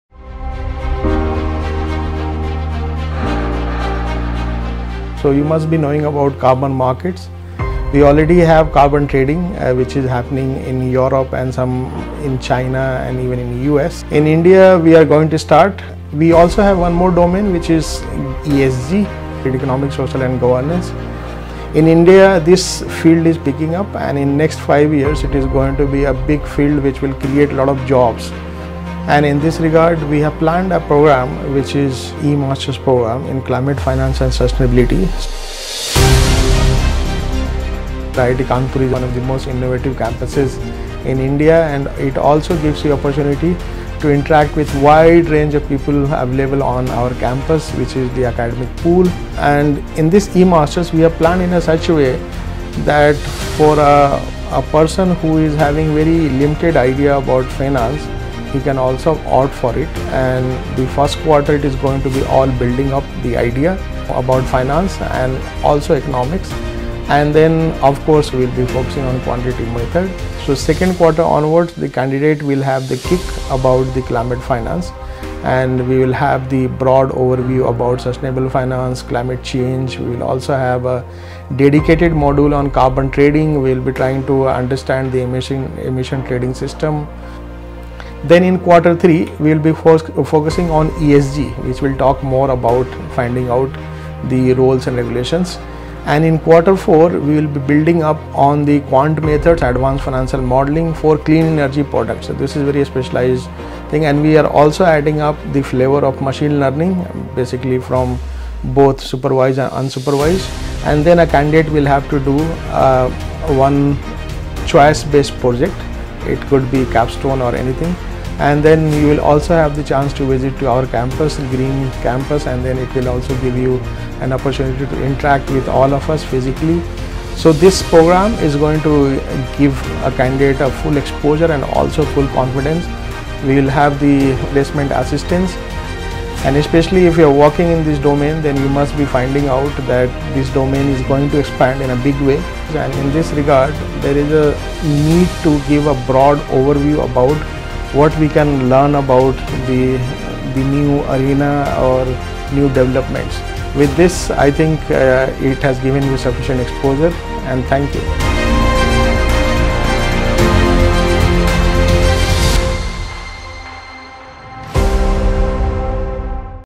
IIT Kanpur | e-Masters in Climate Finance and Sustainability | Programme Intro
Watch Dr. Wasim Ahmad, Associate Professor, Economic Sciences at IIT Kanpur, discusses carbon trading, ESG and its opportunities in the next 5 years, and in this regard IIT Kanpur has come up with a unique e-Masters Degree in Climate Finance and Sustainability program.

The e-Masters Degree in Climate Finance and Sustainability course is designed to provide in-depth training and exposure to carbon management, ESG, and green finance over one to three years, a fully online flexible programme. The flexible curriculum and self-paced learning opportunities are salient features suitable for working professionals. The live and interactive sessions are highly useful for engaging in discussion and practical learning.

eMasters Degree, offered by the Indian Institute of Technology Kanpur (IIT Kanpur), are degree credential programs with industry-focused areas that are highly aspirational for working professionals. With eMasters working professionals get an opportunity to upskill themselves in their career with a prestigious institute like IIT Kanpur.

eMasters in Climate Finance and Sustainability, focussing on carbon management and sustainable finance also offers an opportunity to be part of IIT Kanpur’s active academic environment through a campus immersion program that includes visits to various facilities, including mentorship for startups and placement assistance.

Key Features:
e-Masters Degree from IIT Kanpur
Learn from the leading research faculty
Receive mentorship, career support from IIT Kanpur placement cell
Incubation support for promising startup initiatives
Become a part of IIT Kanpur's alumni network